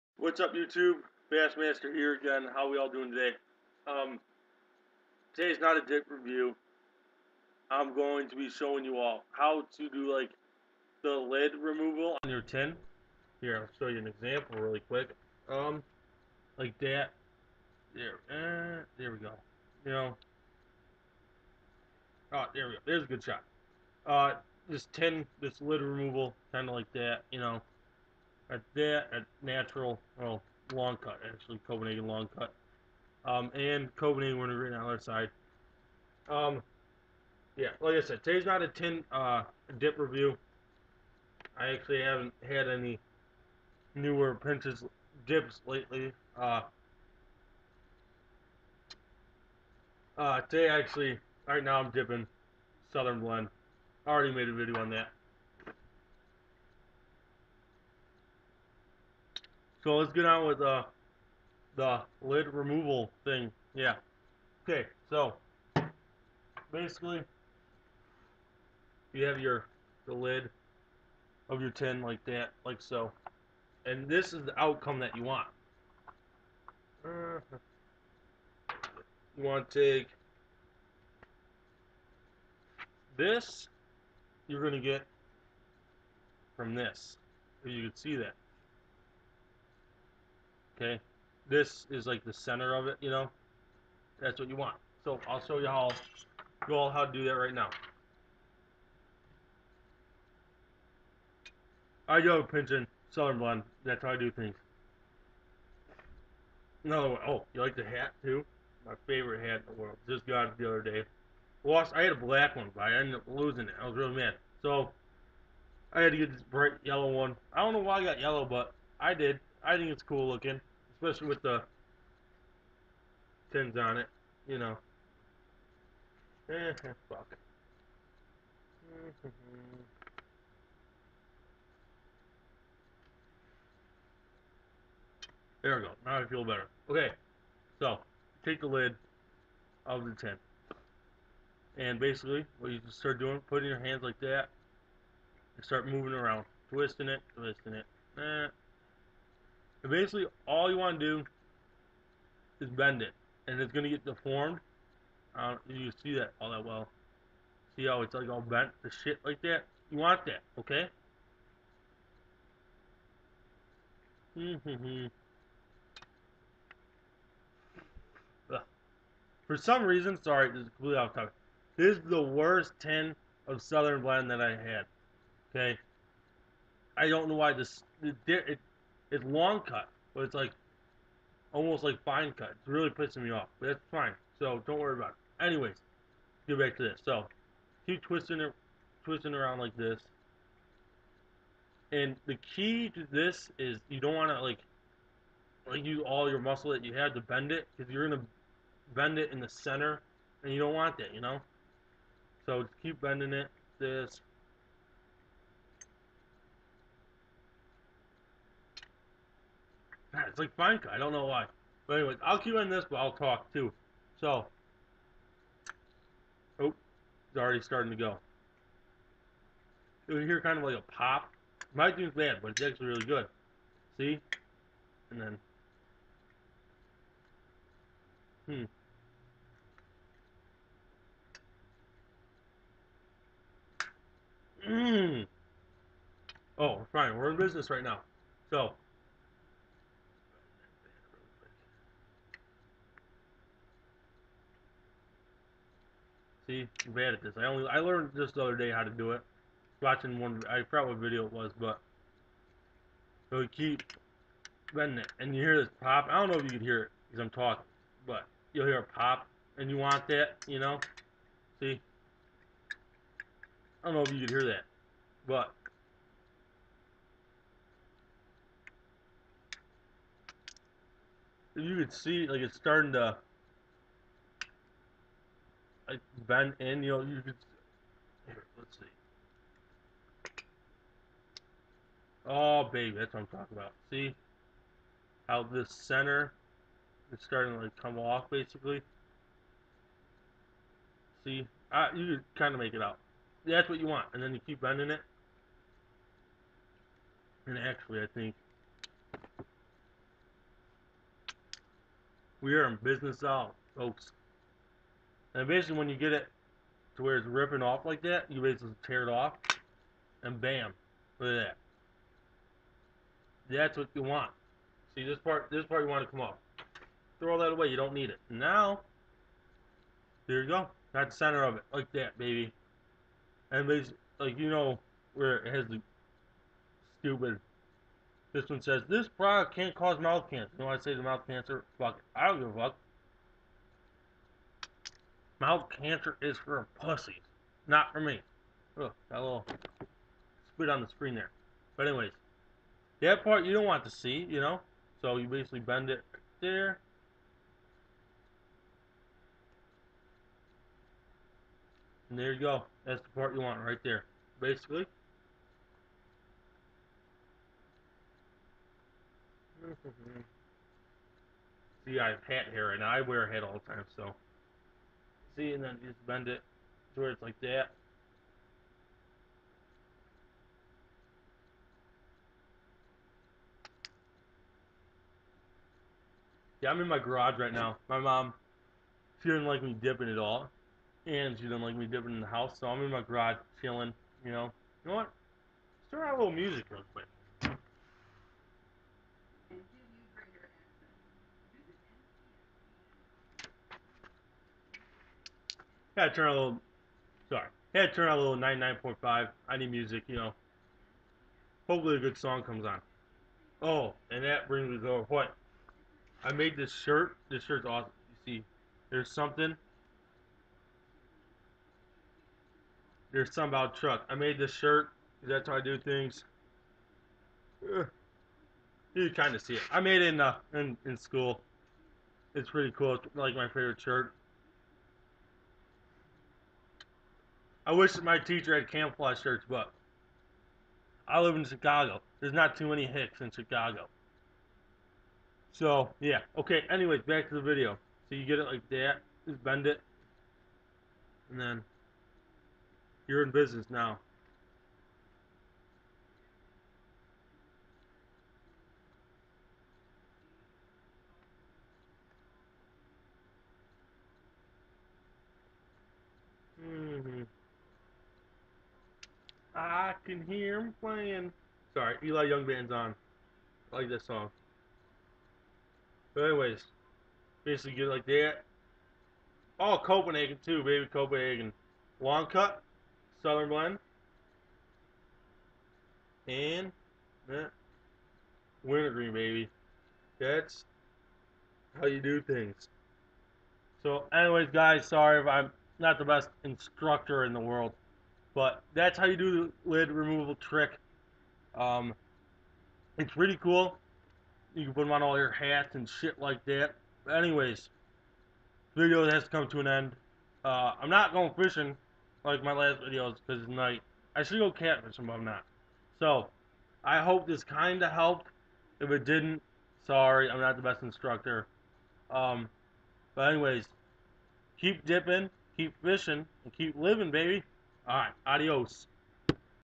Just another Dip Video!!!
this is just a video on how to take the lid of your tin and be able to bend it to were you are able to put it on your hat or what have you. its a really cool trick to learn!